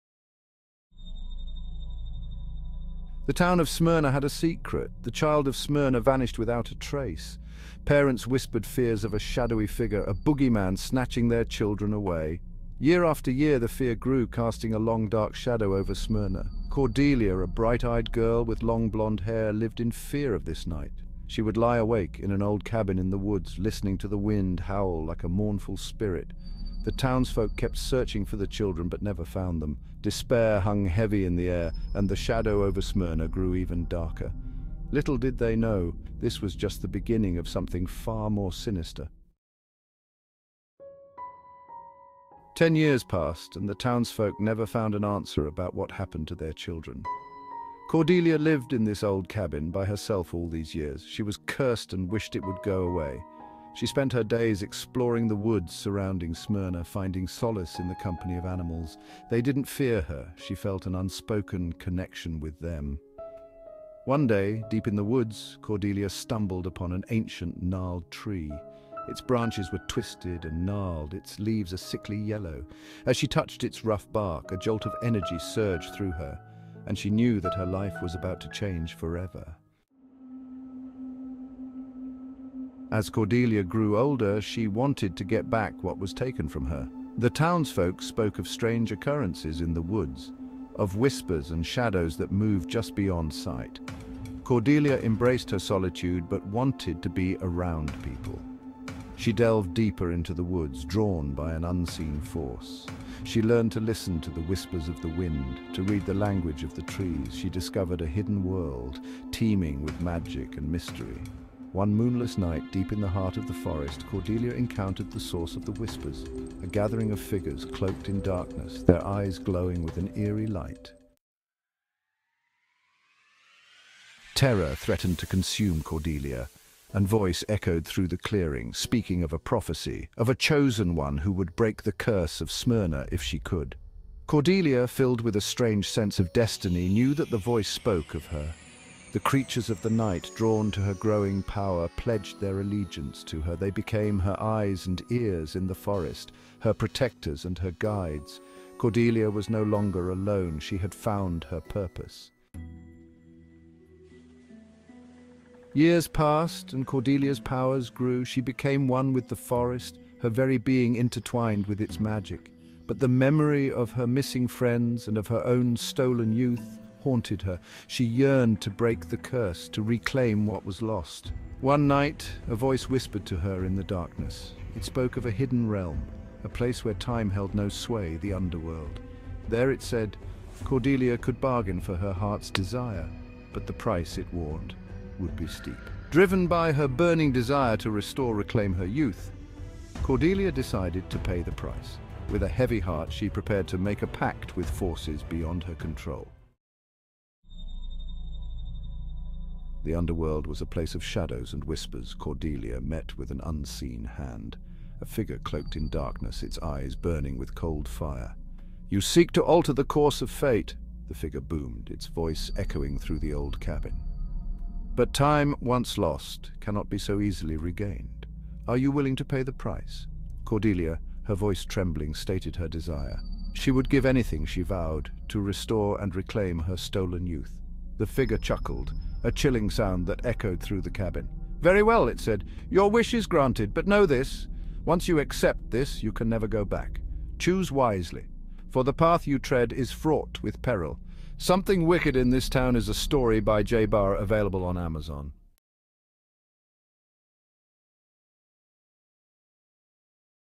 Something Wicked in This Town
I was experimenting with some AI generators, trying to come up with a video for my newest book release, Something Wicked in This Town. It didn't come out exactly as I had hoped but I'm going to go with it anyway. By book can be found on Amazon.